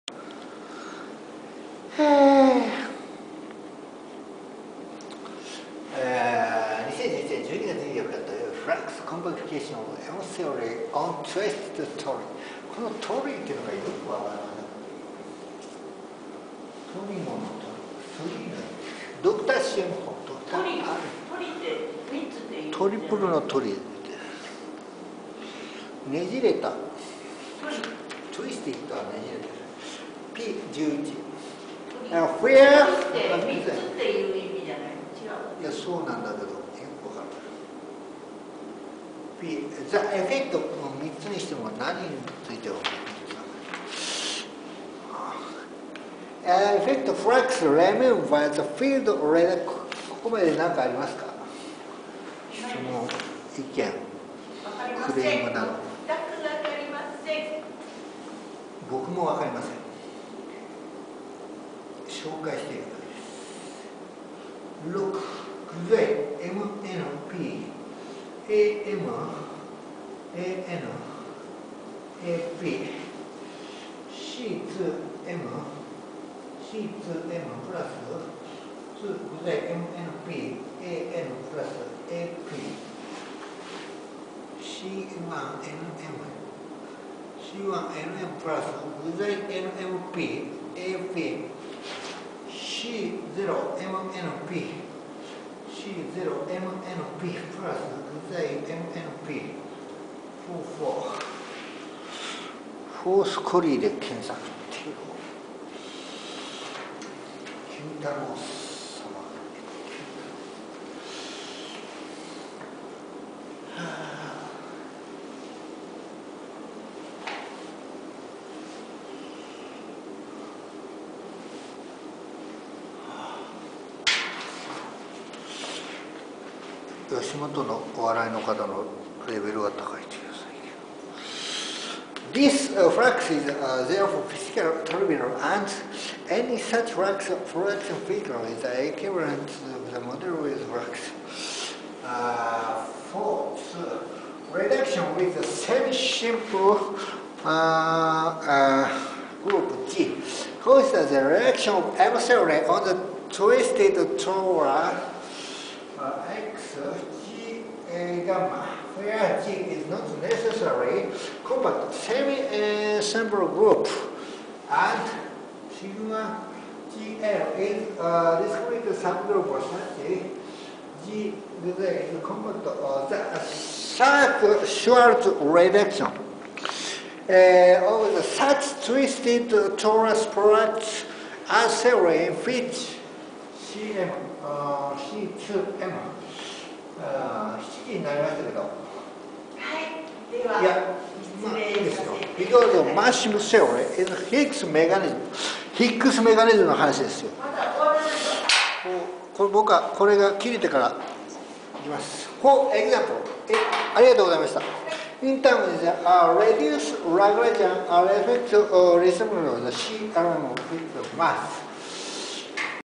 Flux Compactifications of M-Theory on Twisted Tori Authors: Dr.C.M. Hull, MOV04182.MPG
Flux Compactifications of M-Theory on Twisted Tori
Authors: C.M. Hull, R.A. Reid-Edwards
(Submitted on 12 Mar 2006)